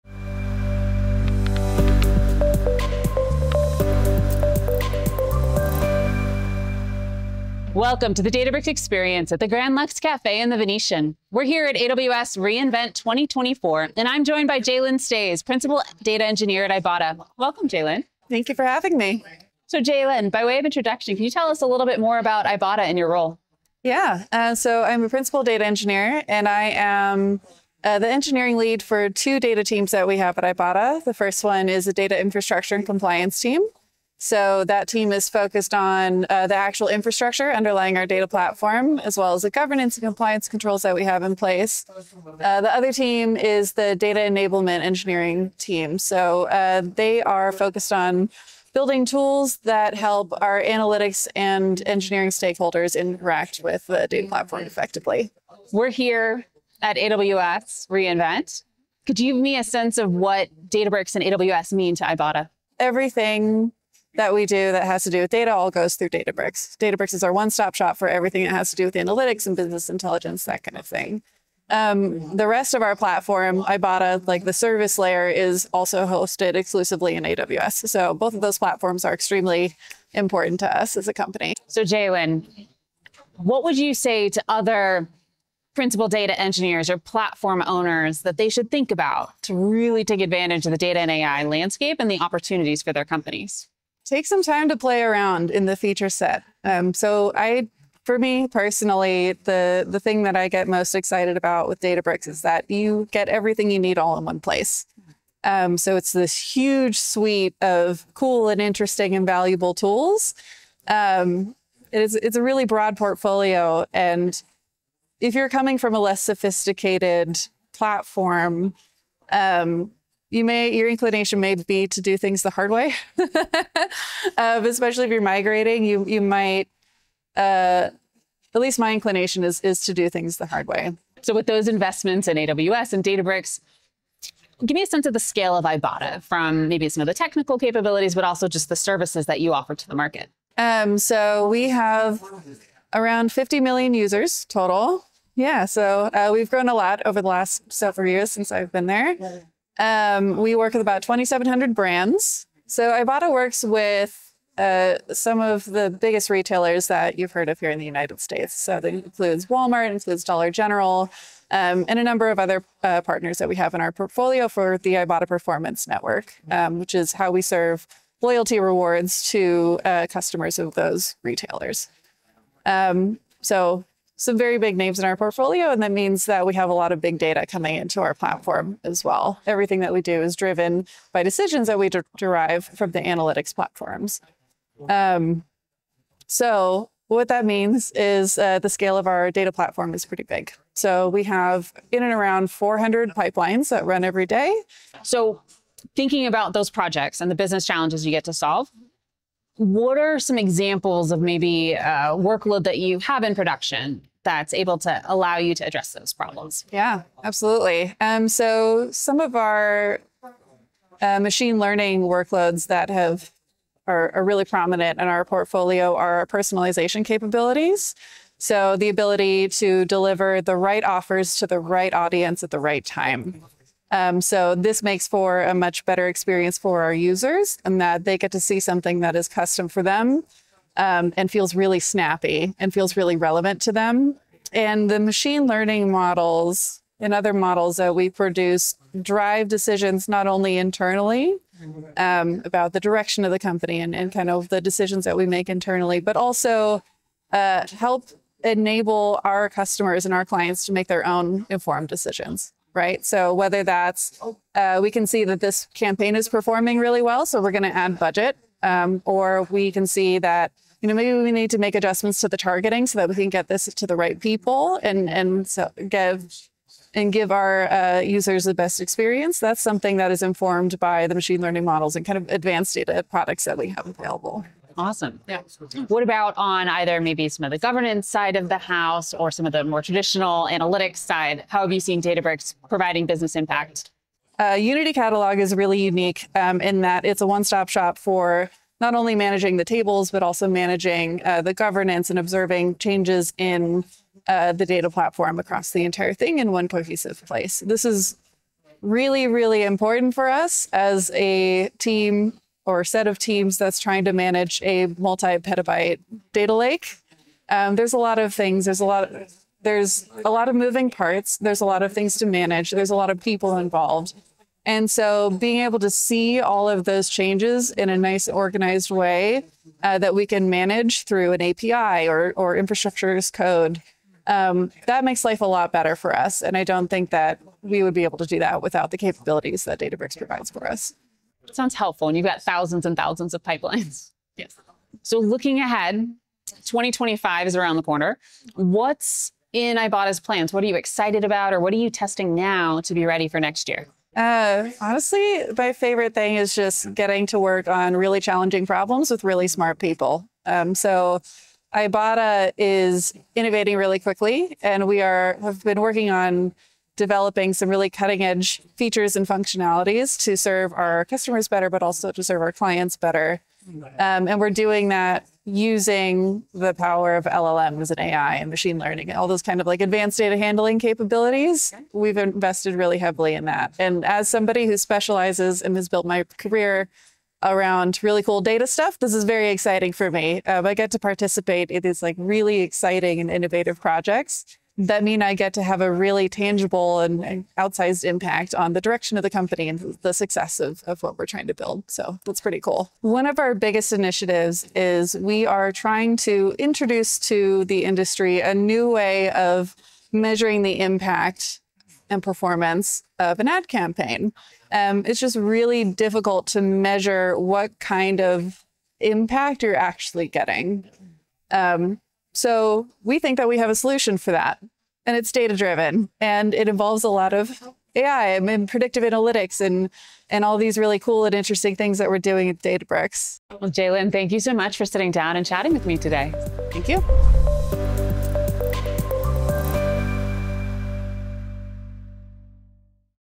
Ibotta Personalized Rewards Innovation with Databricks and AWS
Jaylyn Stoesz, Principal Data Engineer at Ibotta, shares how Databricks and AWS power their data platform to deliver personalized loyalty rewards for over 50 million users. Learn how Ibotta manages a multi-petabyte data lake, builds personalized user experiences and drives business decisions with advanced analytics and machine learning.

Watch now to see how Ibotta is transforming loyalty rewards and customer experiences with Databricks and AWS!